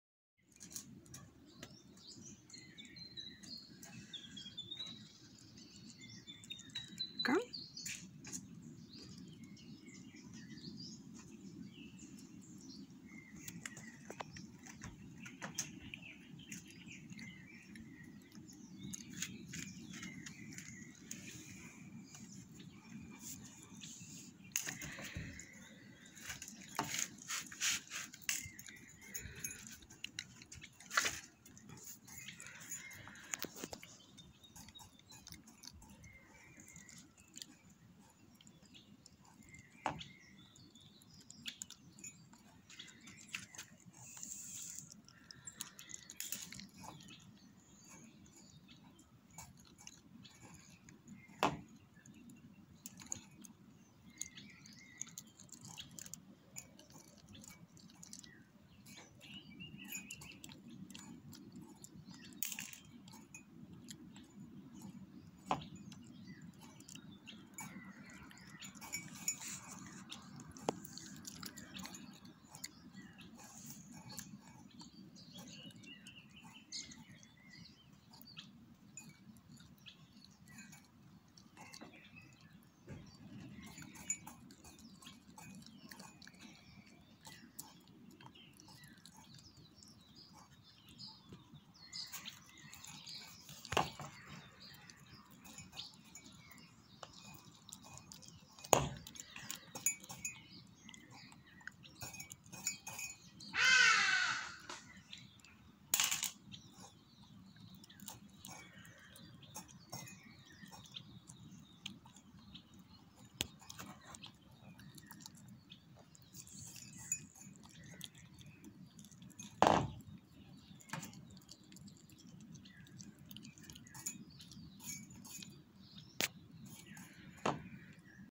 Nellie's Millie Maize Crunch Breakfast ASMR
Another bright South African day has come for us with our half tame monkey Nellie. And we are looking forward to experience some funny and intimate moments with her. Everyday holds a little Nellie surprise.
The sun was up at 5 o clock this morning and Nellie was already waiting for us on the tin roof.
She loves maize but not all sorts of them. The selection process to find the right ones may take some time.
She also loves to climb onto the bird feeders and swing on them while eating the seeds.